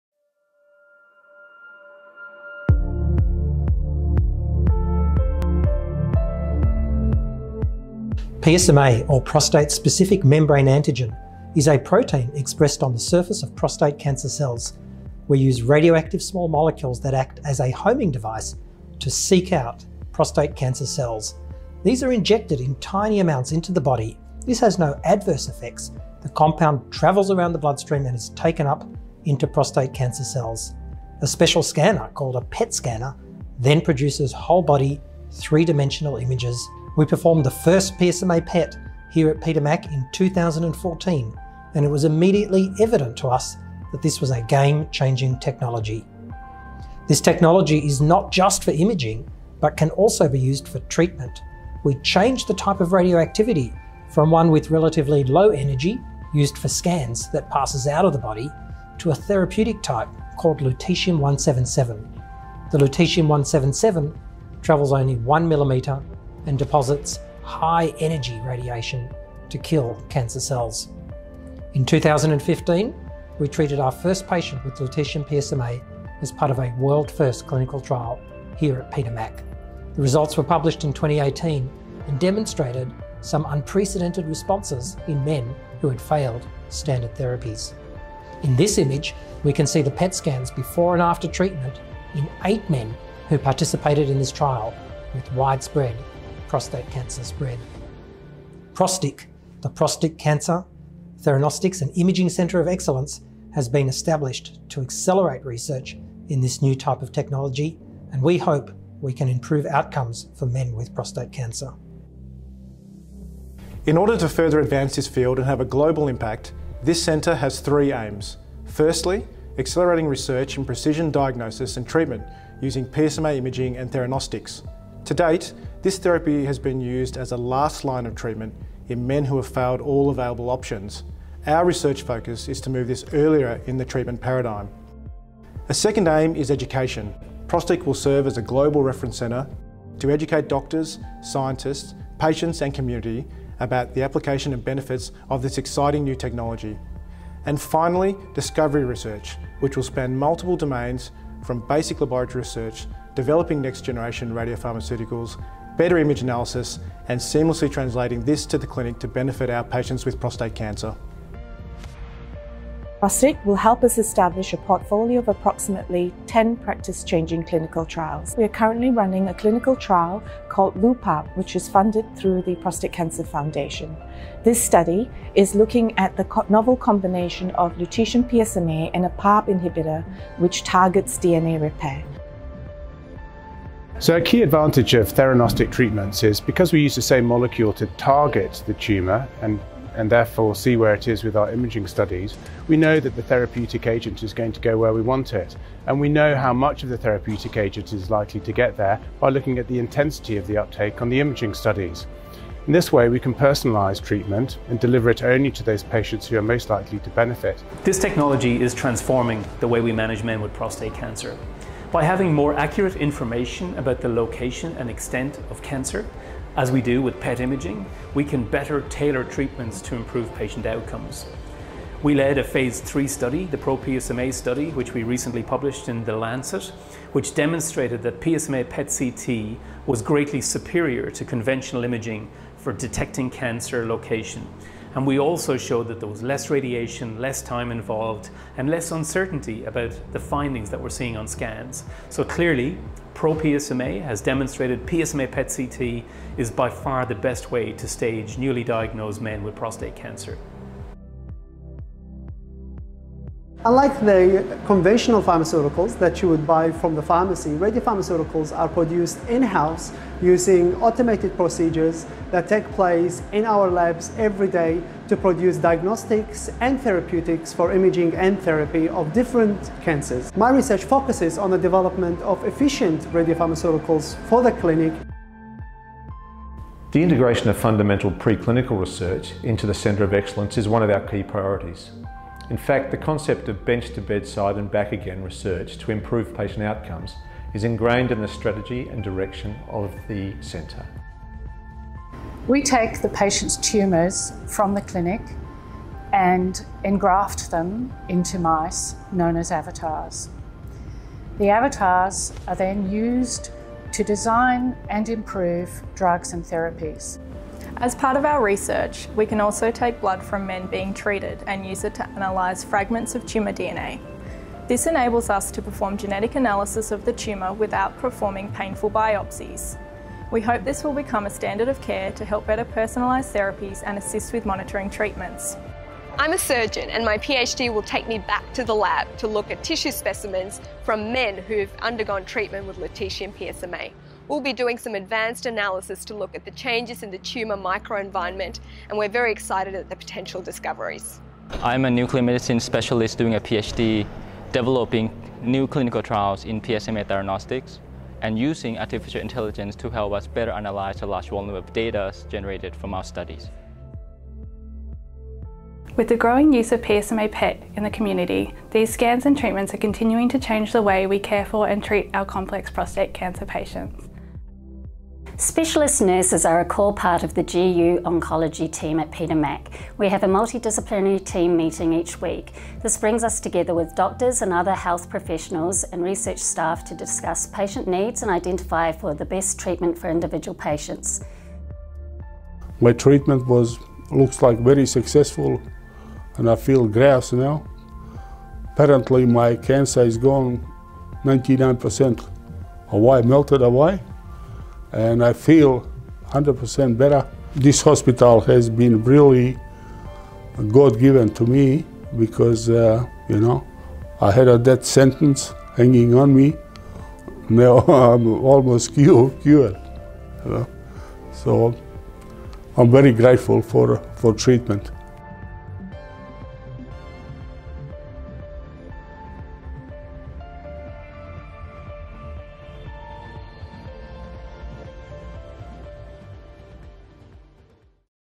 Prostate Theranostics and Imaging Centre of Excellence
The centre of excellence is funded by a grant from the Prostate Cancer Foundation (PCF) and comprises a multi-disciplinary team including nuclear medicine, medical oncology, radiation oncology, urology and laboratory-based doctors and researches with a strong patient-centred philosophy.

ProsTIC performs state-of-the-art research to deliver new paradigms for treatment of prostate cancer driven by seamless integration of clinical trials, pre-clinical and discovery research.

For more, see Petermac.org/ProsTIC